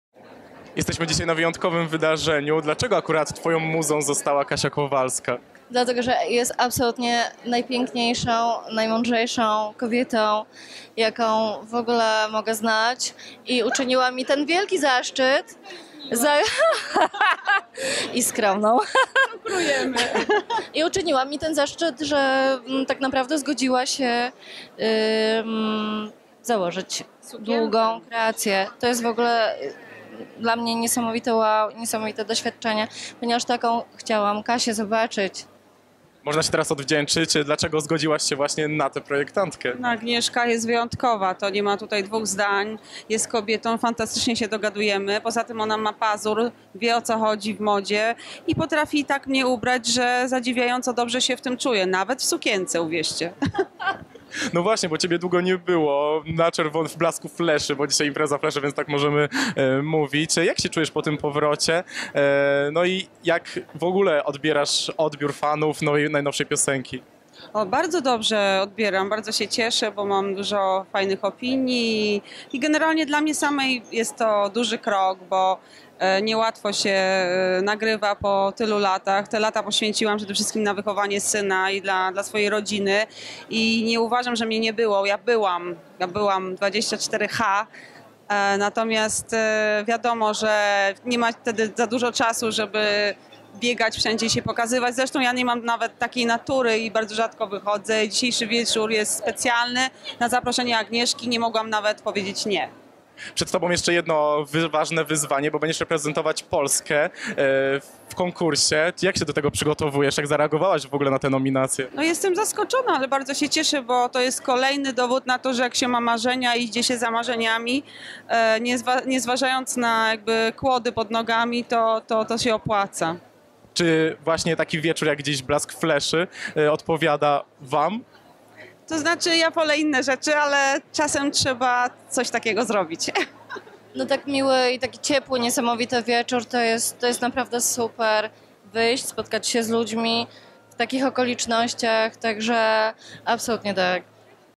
Kasia Kowalska o powrocie do muzyki | PrzeAmbitni.pl
Wywiad z Kasią Kowalską i Agnieszką Maciejak podczas Flesz Fashion NIght
6 września 2016 r.